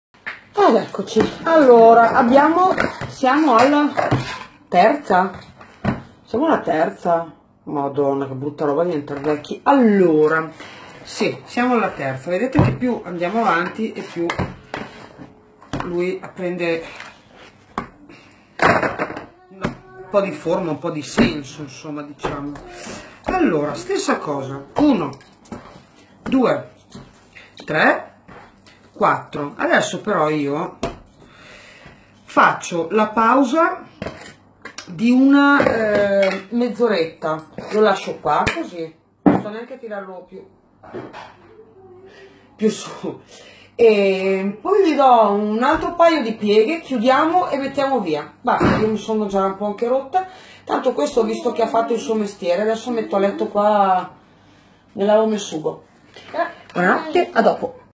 Come fare la Pizza in teglia alla romana - Metodo No Knead 4° - La terza piega
pizza, teglia, pizza in teglia, pizza romana, ricetta, come fare